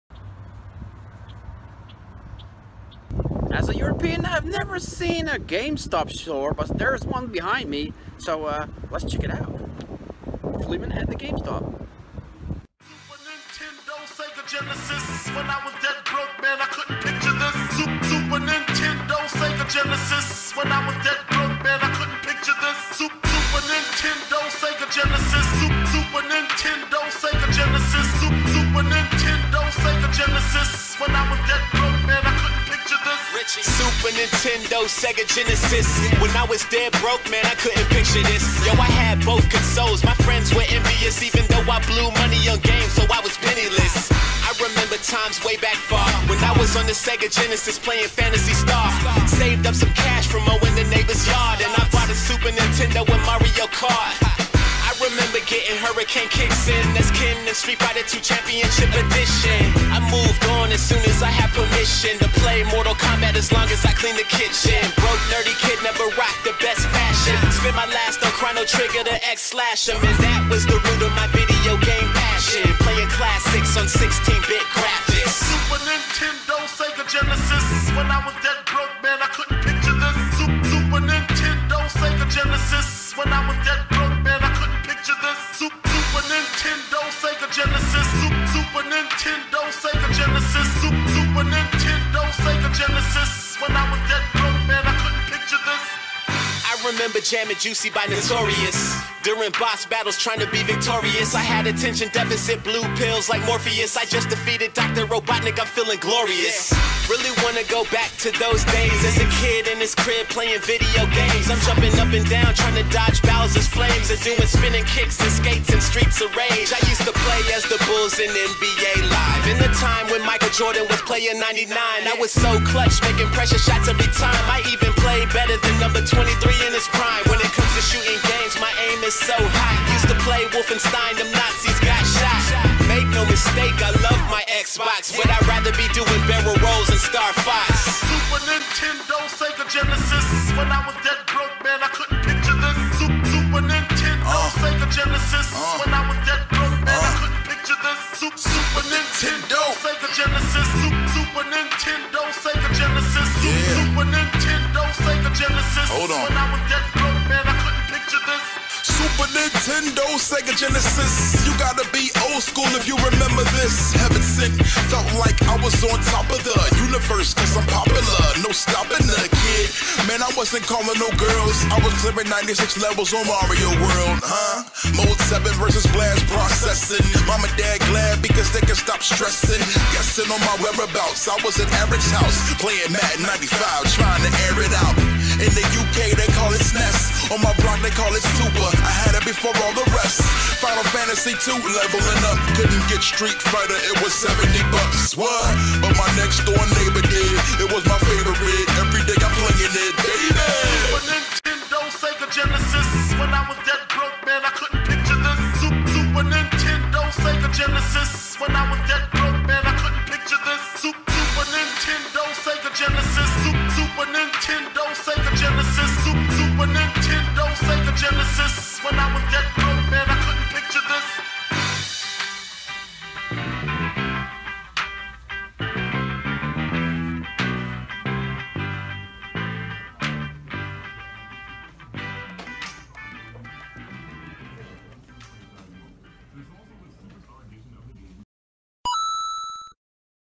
GameStop STORE TOUR
GameStop is in deep trouble. The company is shutting down stores and there are rumours that the end is near.
As a European i only know the gamestop store from YouTube stories. During my American roadtrip i visited a GameStop store.
This virtual store tour might be the only way to visit a gamestop store in the future.